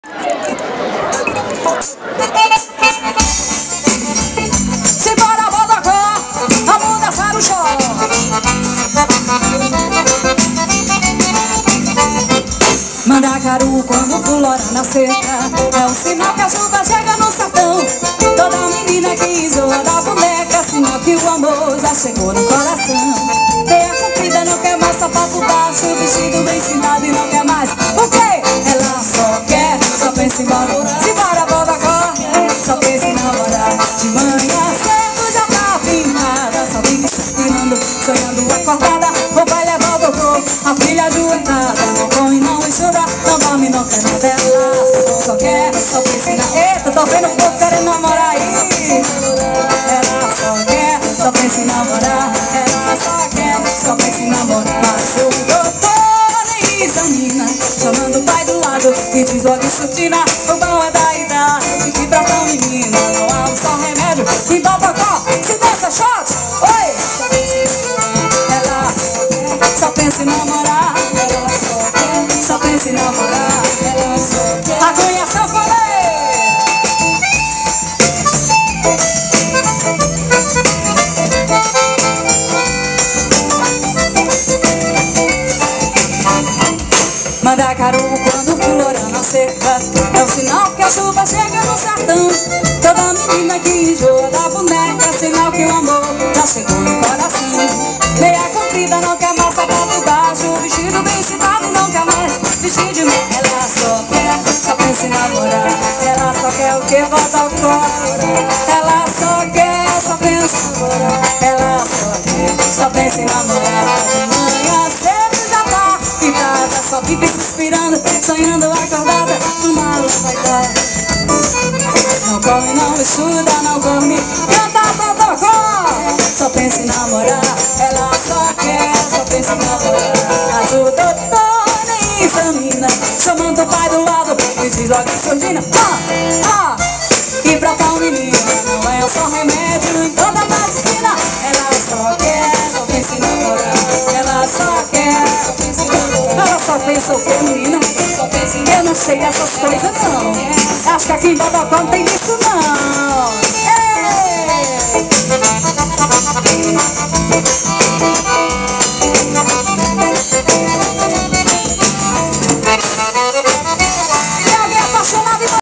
Ana Paula Nogueira em Bodocó-PE18/06/2016
Ana Paula Nogueira contagiou o público com muito forró pé de serra neste dia 18 de junho em show de Flávio Leandro em Bodocó